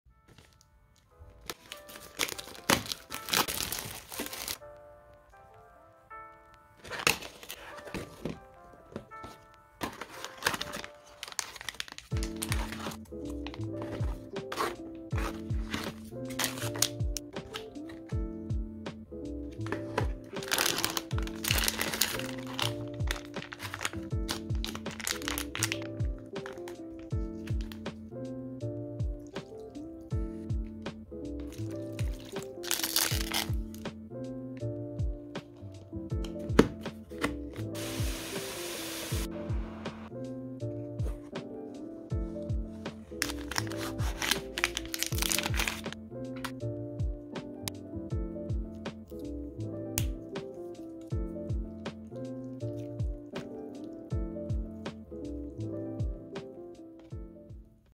Trying out a SELF-HEATING vegetable hot pot! 🍜
Trying out a self-heating vegetable hot pot for the first time! 10/10 would buy it again 😁